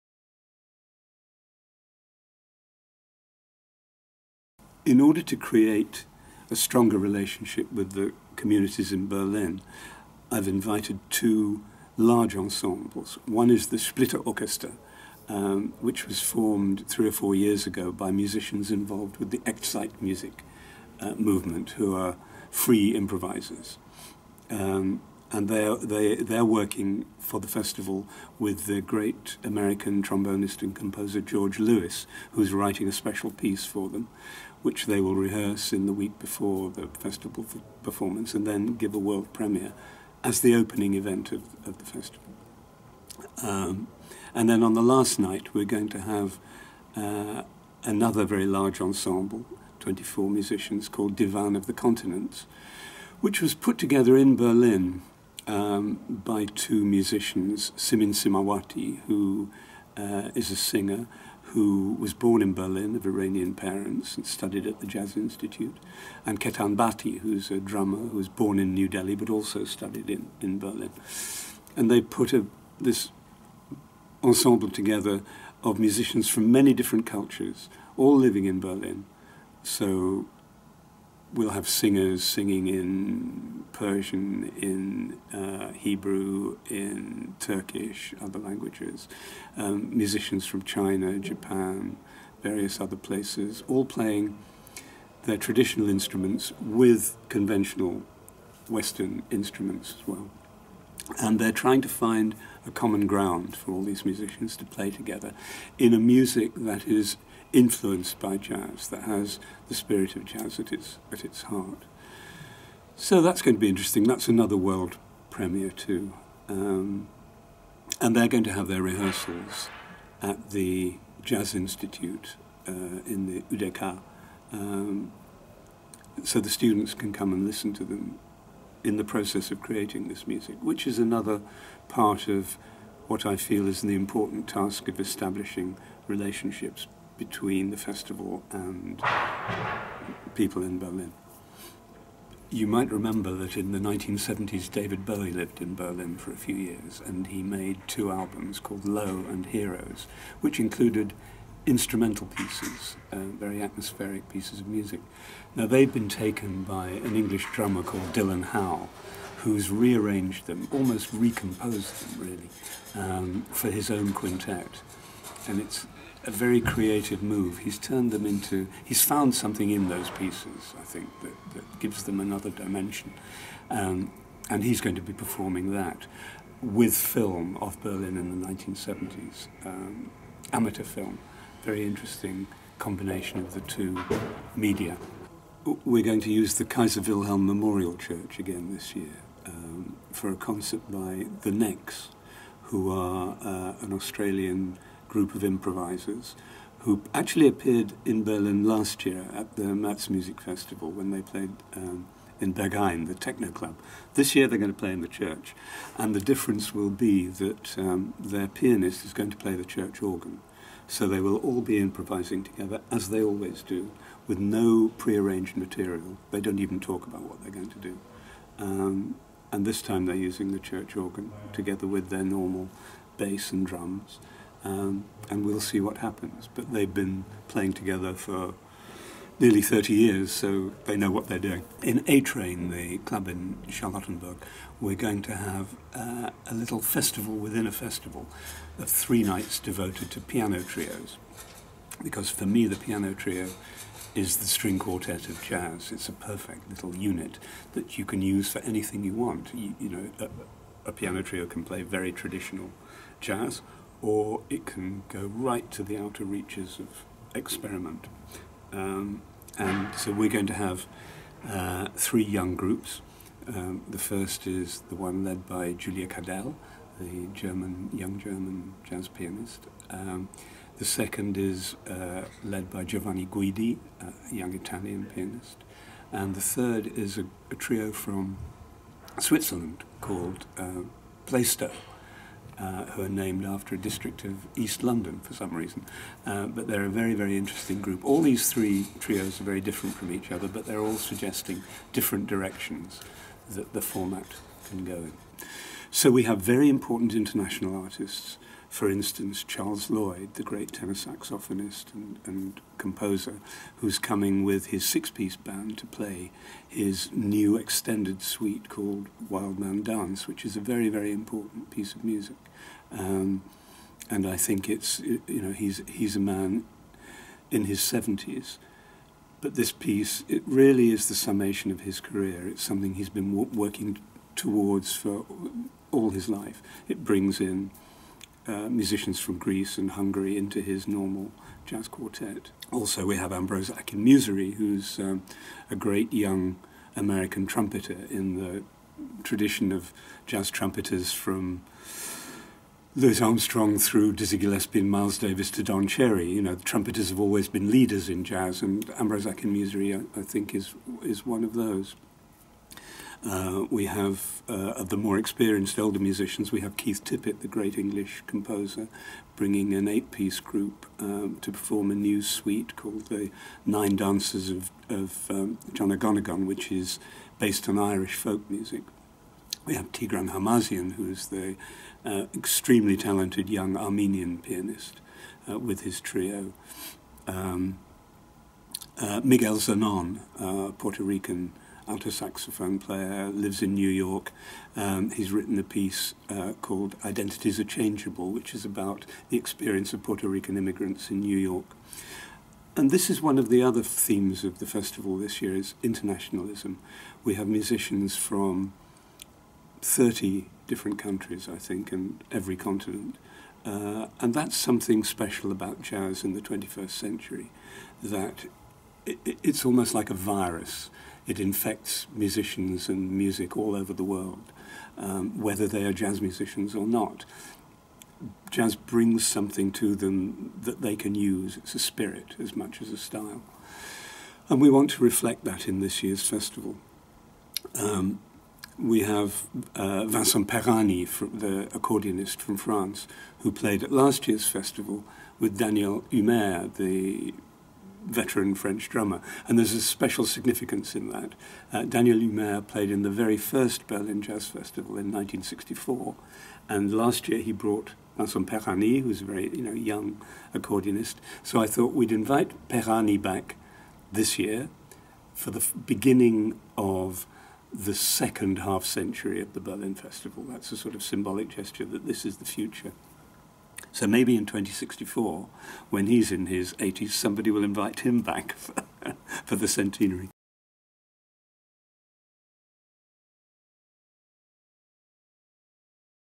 Richard Williams über das Jazzfest Berlin 2015
Als Berliner Jazztage 1964 gegründet, zählt das Jazzfest Berlin zu Europas ältesten und renommiertesten Festivals seiner Art. In diesem Jahr liegt die künstlerische Verantwortung erstmals bei dem britischen Musik-Journalisten Richard Williams. In diesem Video stellt der neue Künstlerische Leiter sein Debüt-Programm vor.